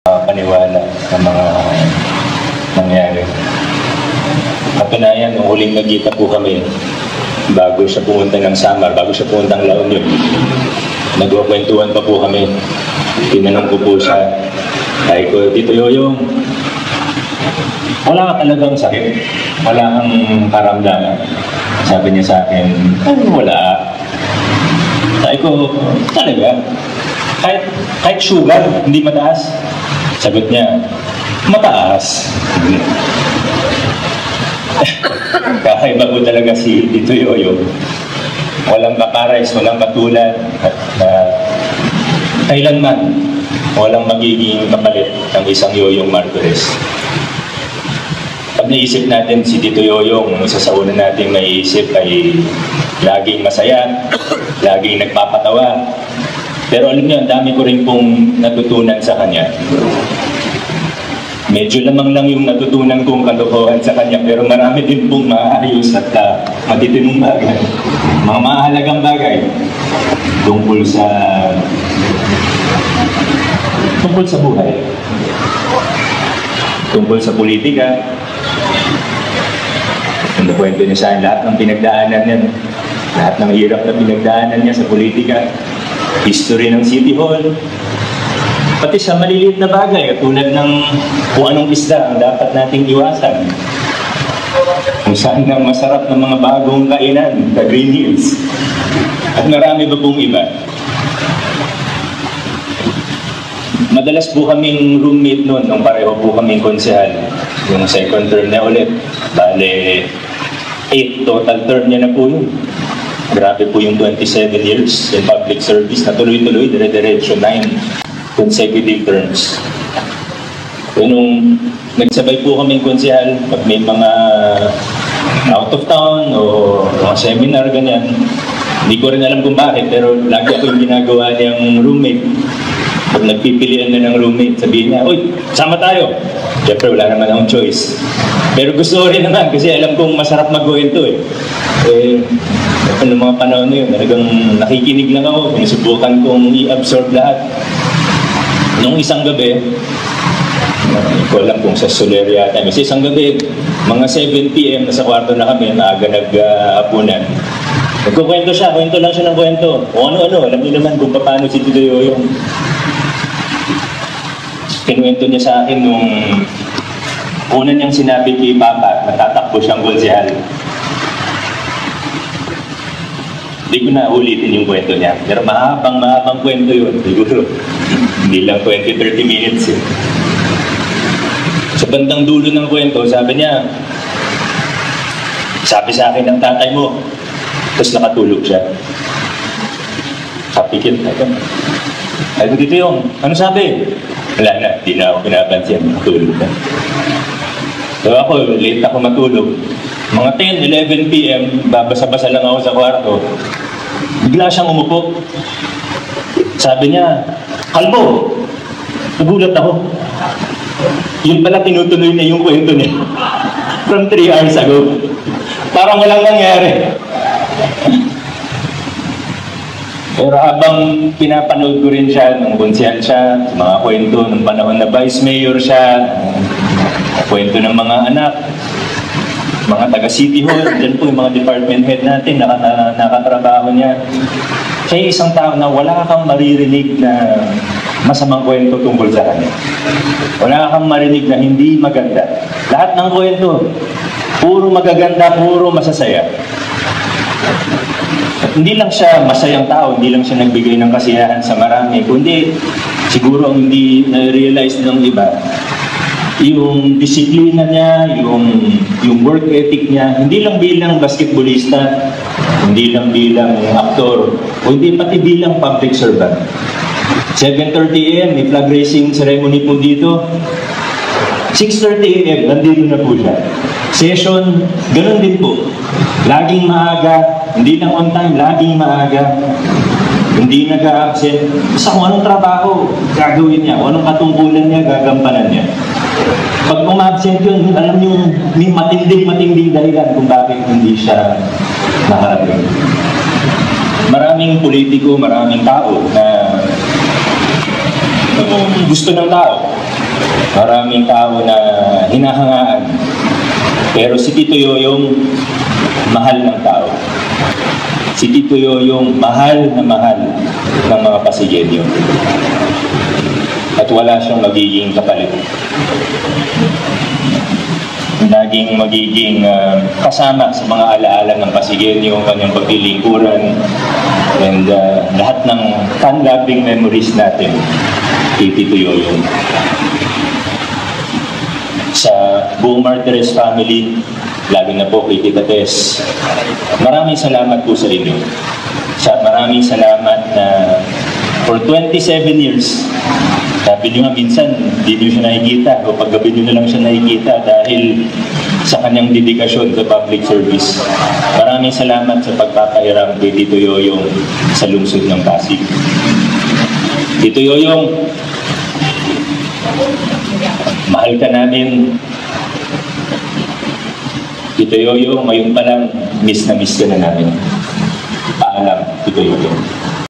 Mayor Vico nagbigay alaala sa pumanaw na si Yoyong Martirez!
Mayor Vico nagbigay pugay sa pumanaw na dating vice mayor ng pasig na si Yoyong Martirez.

video courtesy of INewsPasig